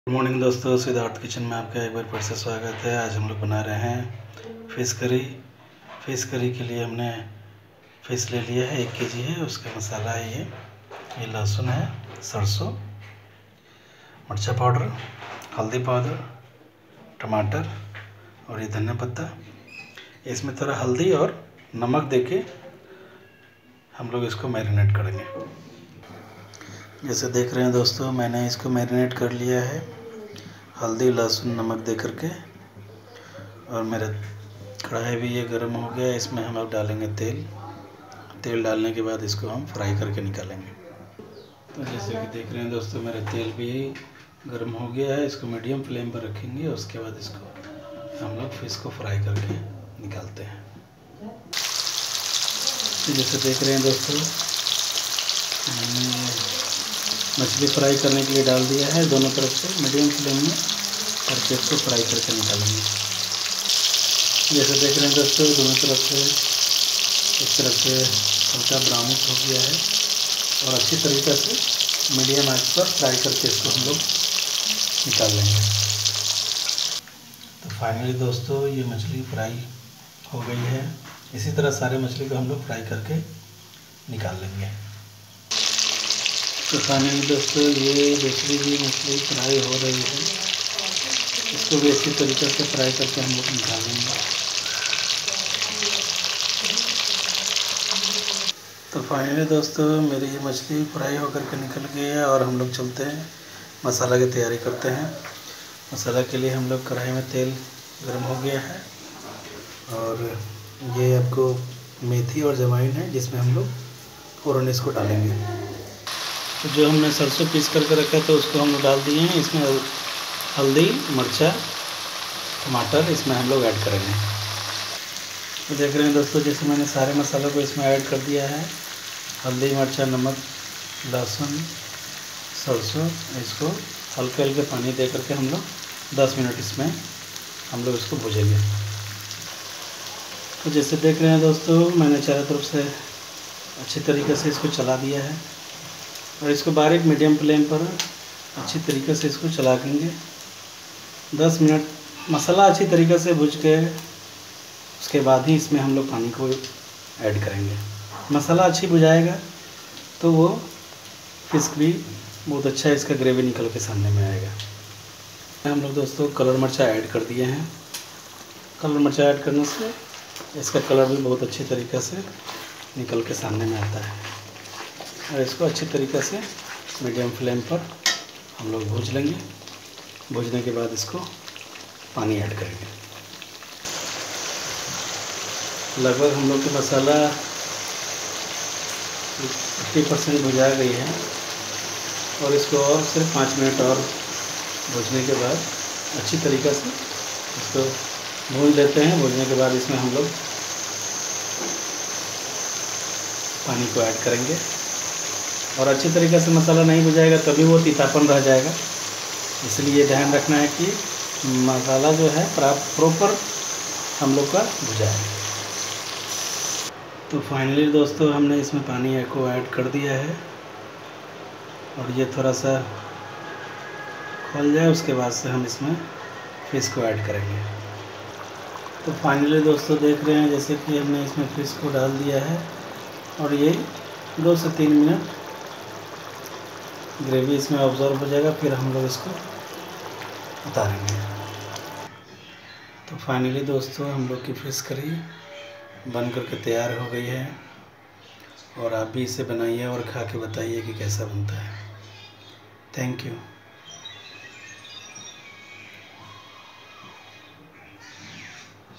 Fish Curry# bihari fish curry kaise banate hai#
Fish Curry Masala#
1.Fish -1kg
2.Sarso sabut-4 tsp big
4.tamoto-1pic
5.methi sabut-1/2tsp
6.Ajwian-1/2tsp
7.dhaniya pata-15g
8.oil-150g
9.kashmri lal mirch-1tsp
10.Haldi power- 1.5tsp
11.mirchi power-2tsp
12.garlic sabut-2pic
13.salt-swad ke hisab se
14.kasturi methi-1tsp

dosto bihari style me fish curry bana ke dheakyie bhaut swadist lagta hai.batayie ga kaisa laga.